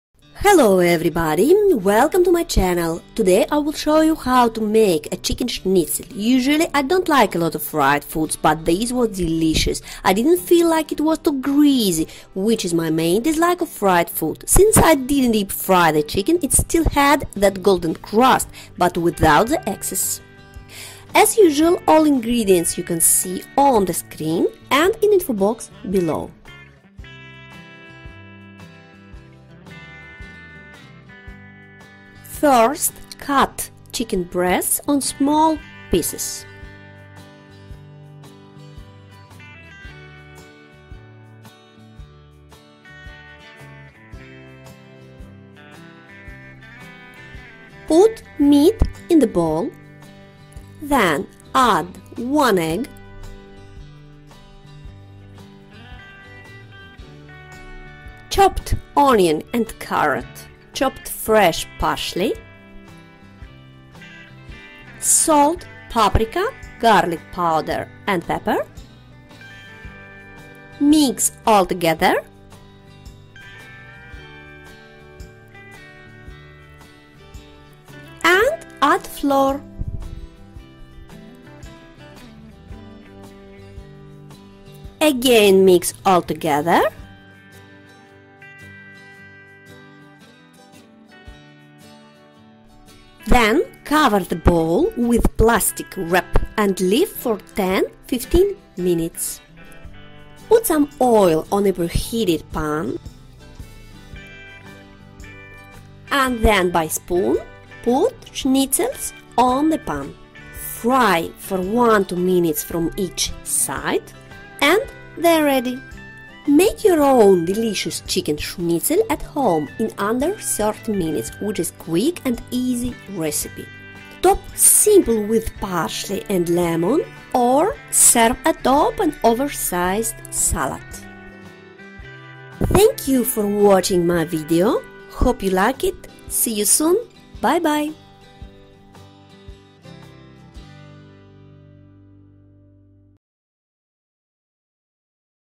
Delicious Chicken Breast Schnitzel - easy recipe with great result!
Make your own tasty chicken schnitzel at home in under 30 minutes with this quick and easy recipe!
This schnitzel is perfect for burgers and sandwiches. Also goes very well with fresh salad, grilled vegetables or riсe.

Ingredients:
chicken breast (fillet) - 300 gram
1 egg
chopped onion - 30 gram
grated carrot - 50 gram
all purpose flour - 3 tablespoons
chopped parsley - 10 gram
salt, paprika, garlic powder, pepper - by taste